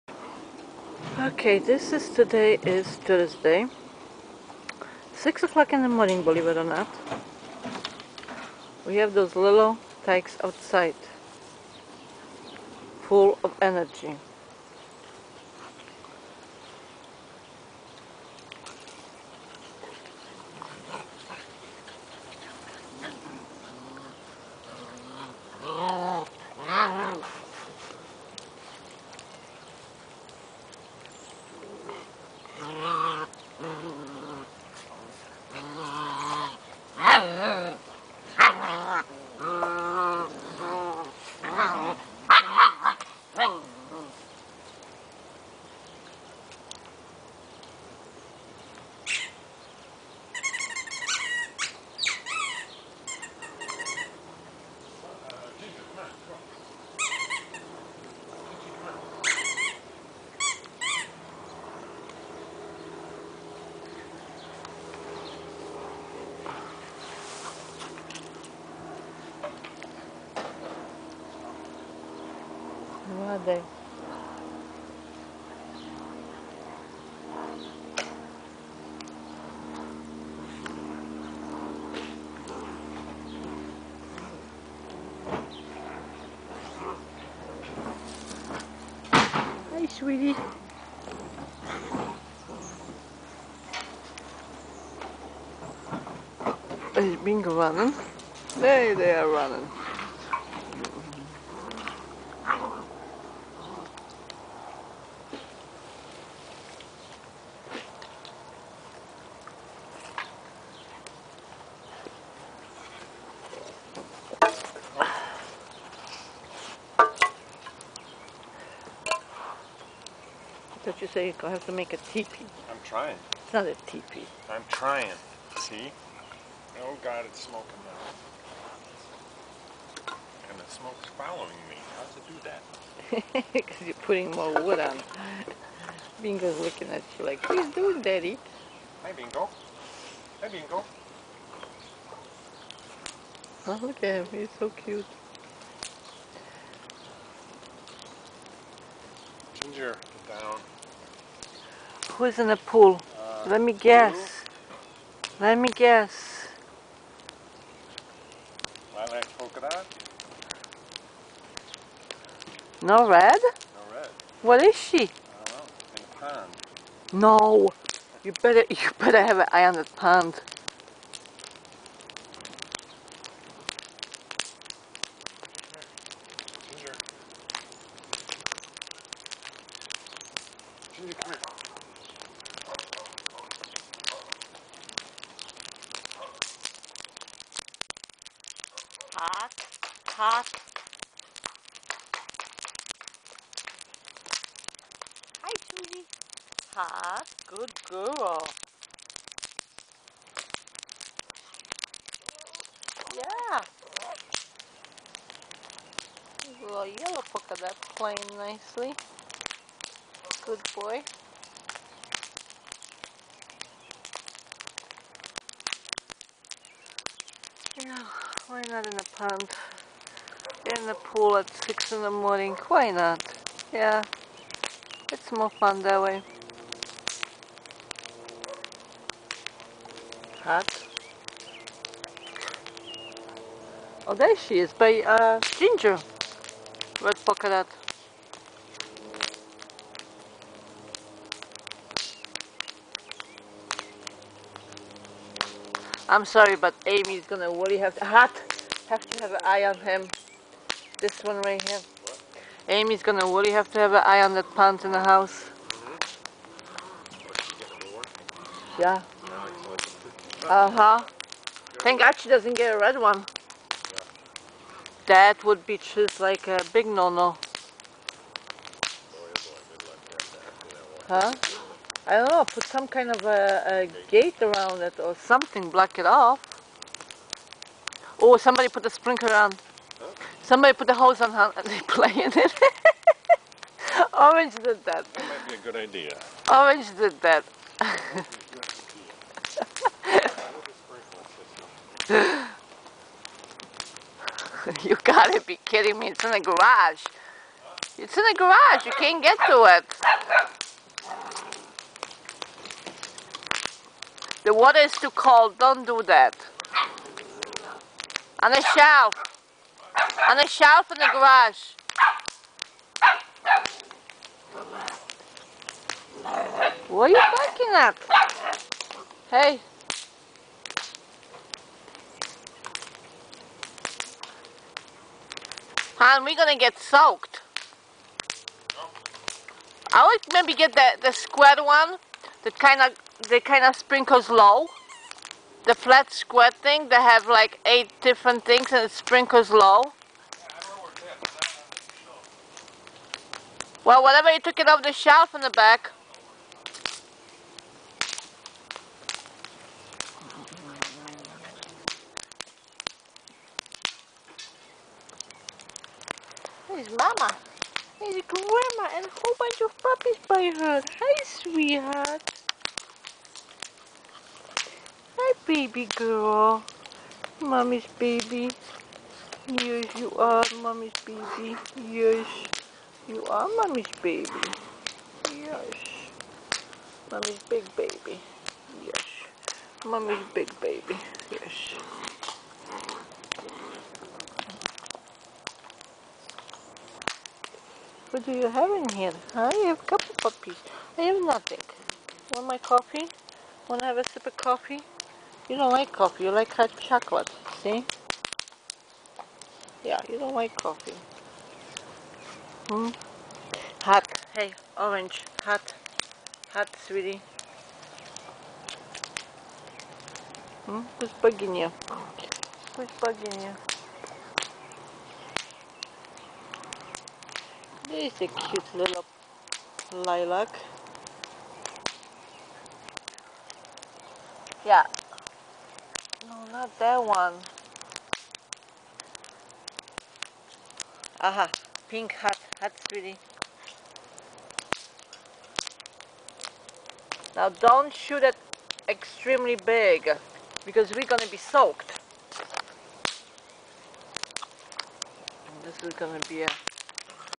Sofie's and Ginger puppies 7 weeks old
Wednesday at 6 AM -  outside in the morning  full of energy to play